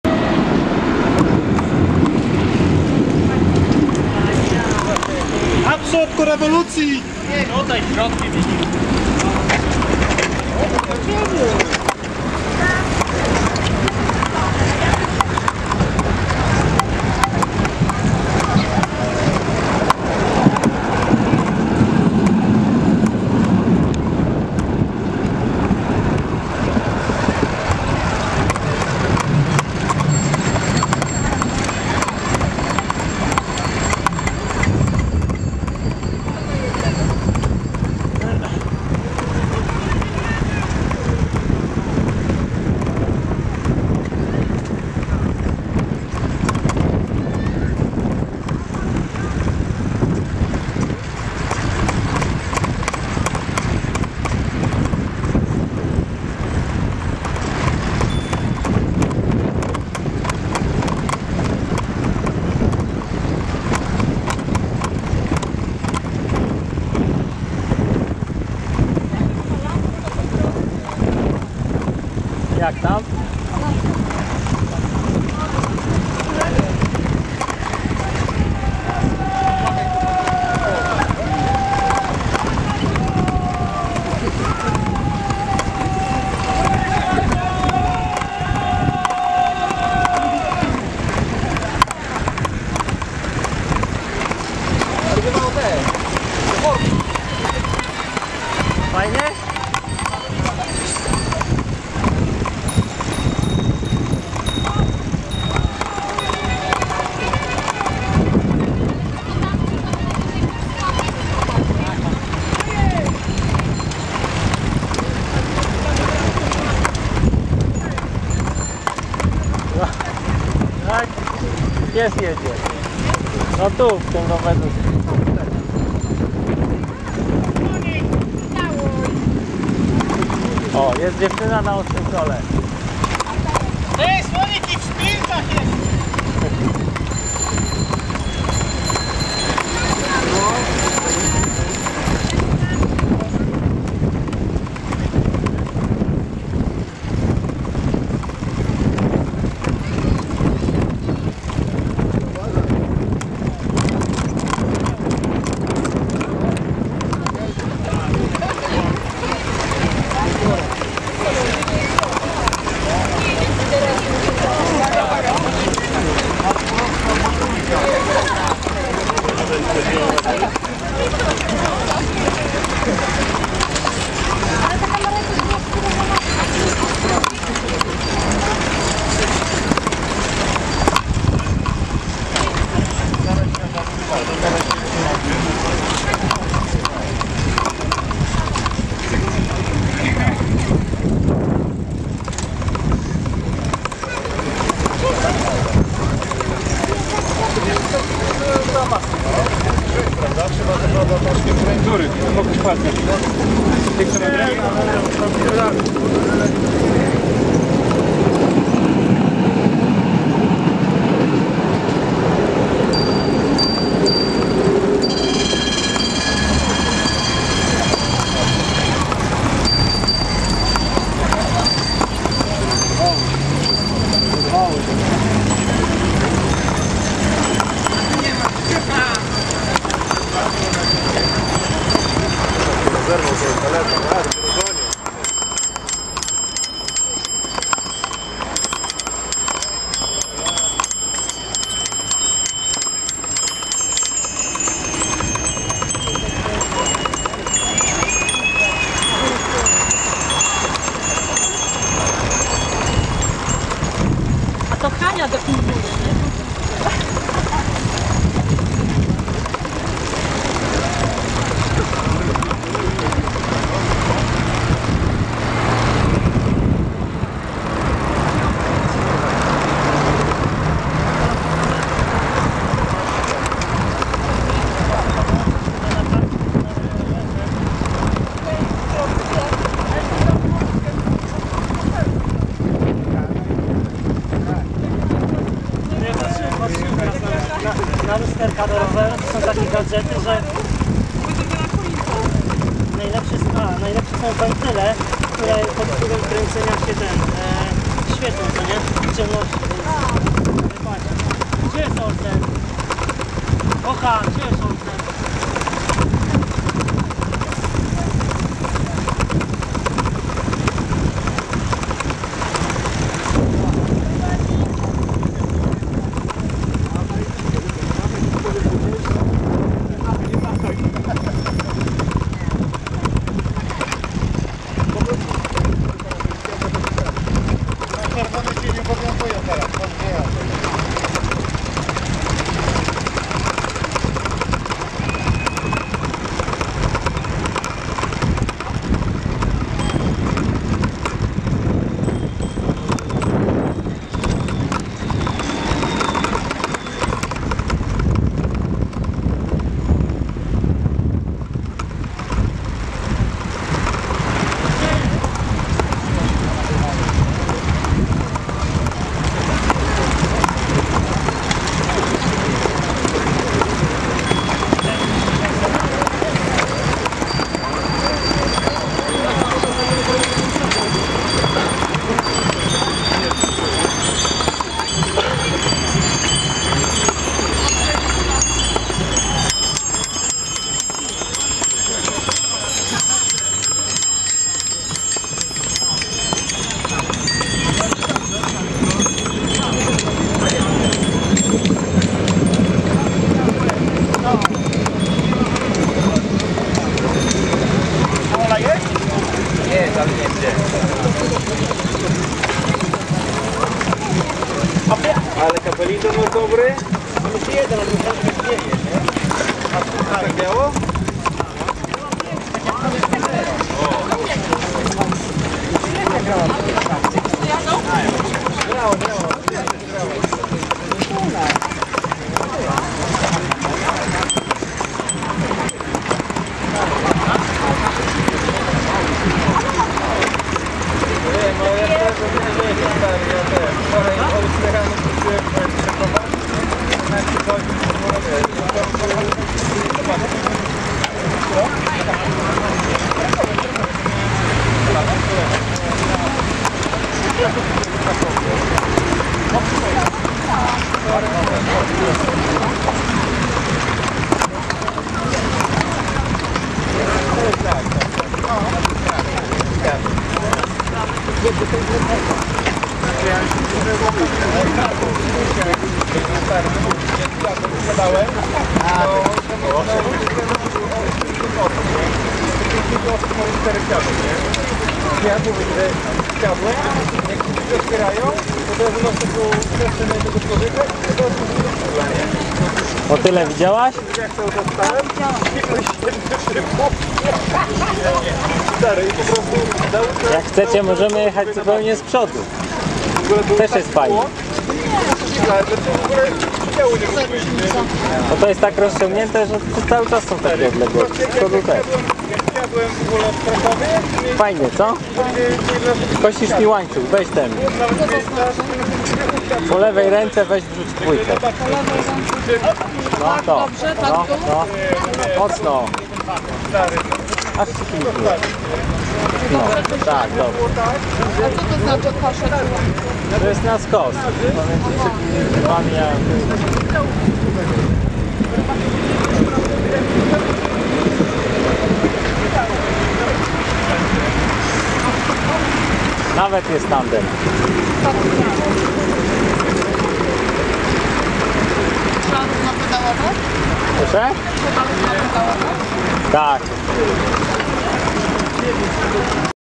20110605 WPR 2011 01
Wielki Przejazd Rowerowy 2011 z perspektywy roweru. Materiał nakręcony aparatem Canon Powershot S3 IS w trybie video. Pierwsze 10 minut, od wyjazdu w Grunwaldzką w Gdańsku Głównym.